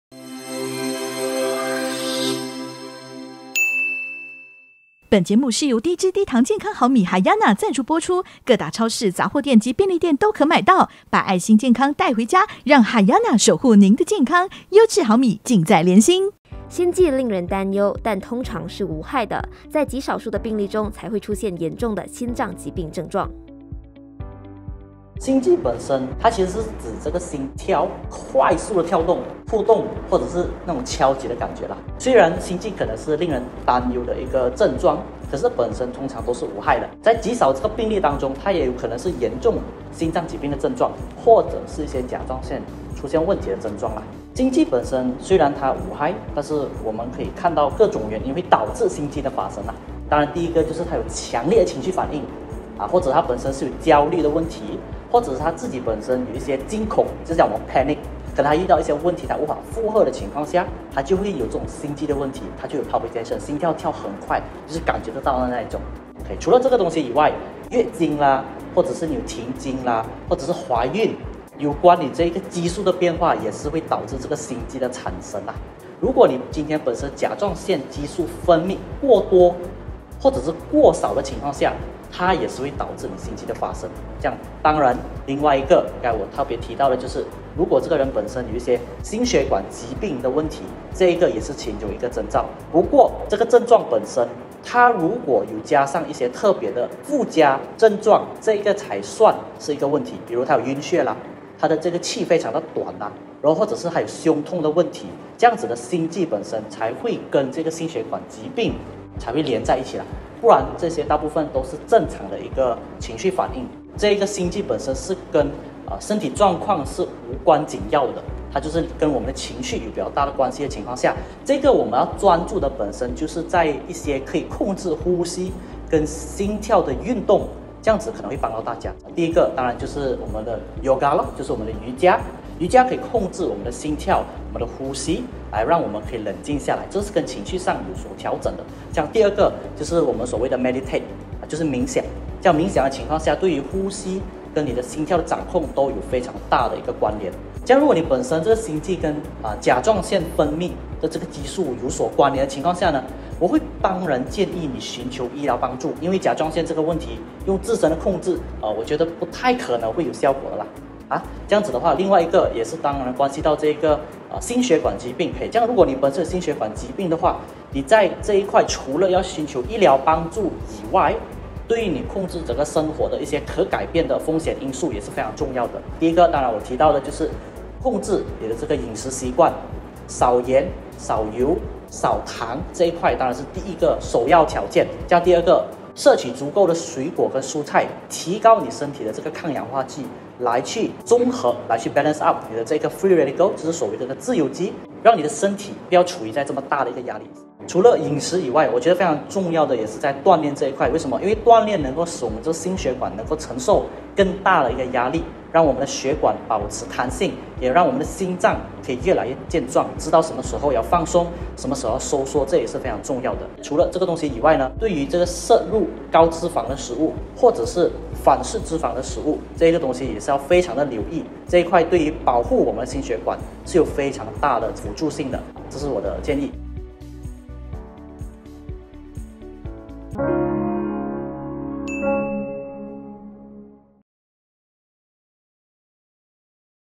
【健康最给力】心悸令人担忧，但通常是无害的
心悸令人担忧，但通常是无害的。
在极少数的病例中，才會出現严重的心脏疾病症状。